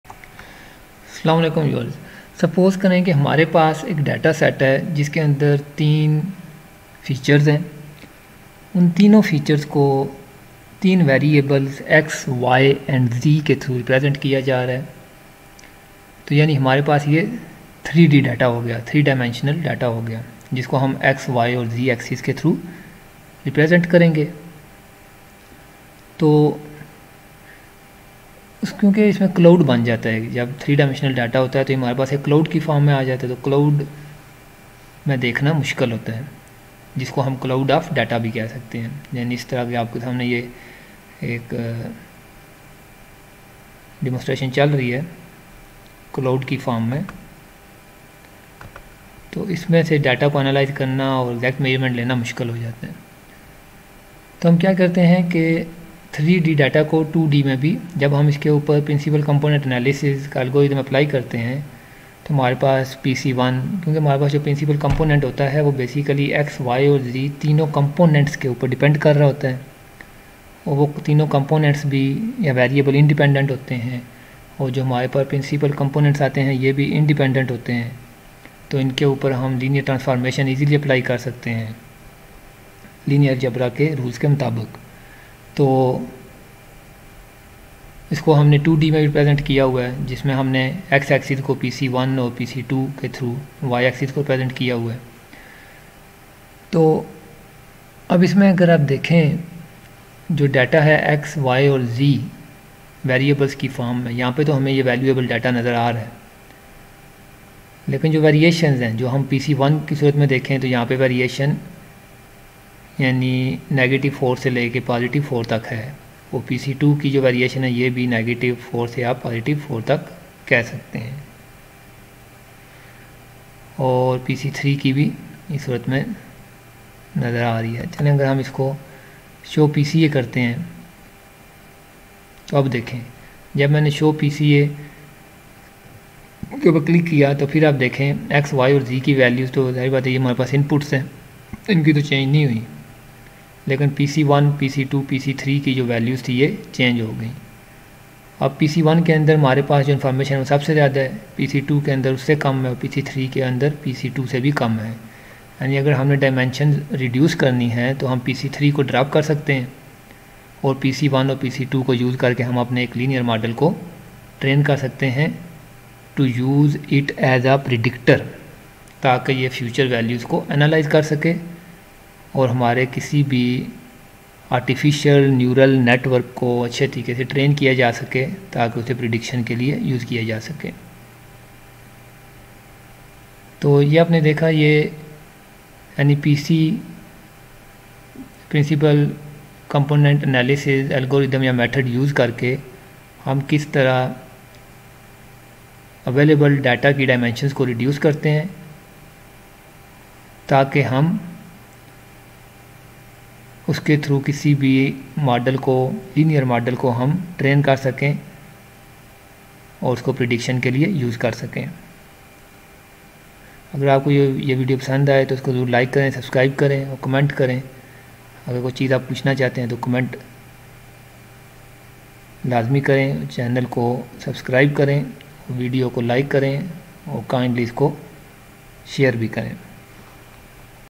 PCA Transformation of 3D data set PCA_Urdu_Part 9.
In this video, we discussed which principal component should be kept and which component must be dropped due to having less information.